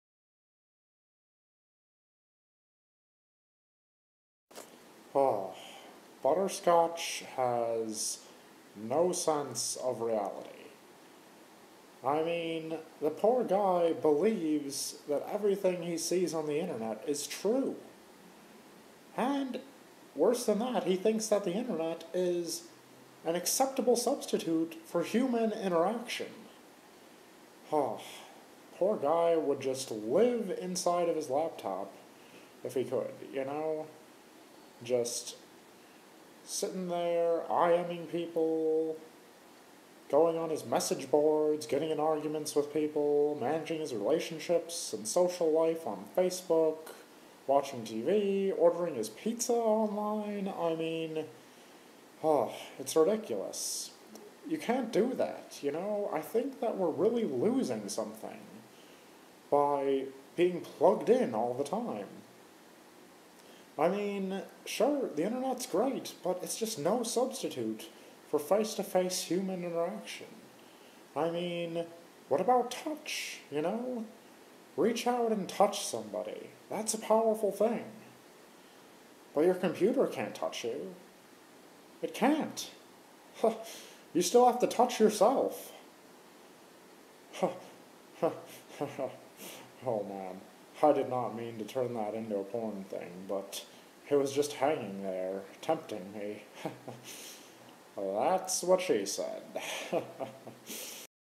Losing The Human Touch
Mel makes his case that the internet is a poor substitute for face-to-face human interaction